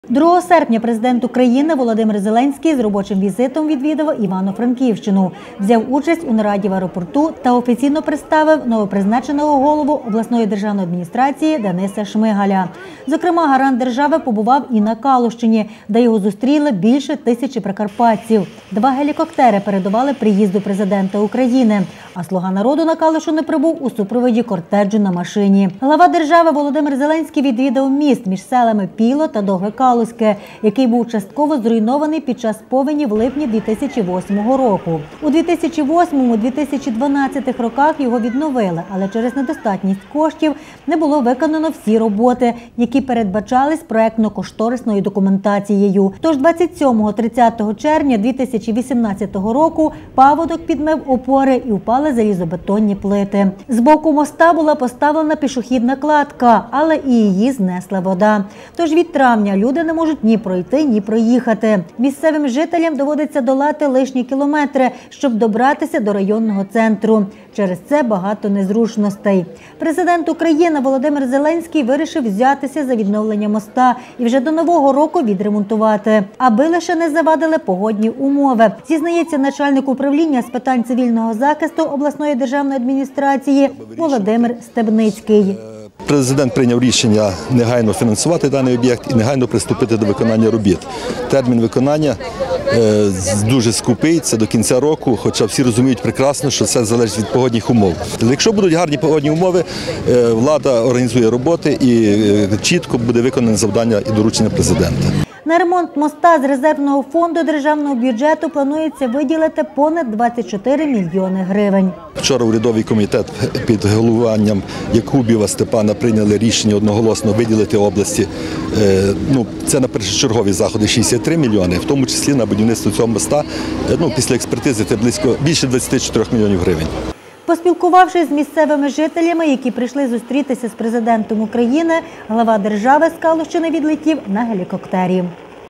Президент Зеленський пообіцяв відбудувати міст між Пійлом і Довгим-Калуським до кінця року
Міст у селі Довге-Калуське таки відбудують до кінця року. За його відновлення пообіцяв взятися Президент України Володимир Зеленський, який відвідав зруйнований міст між Пійлом і Довгим-Калуським. На його ремонт планують виділити більше 24 мільйонів гривень. Детальніше далі.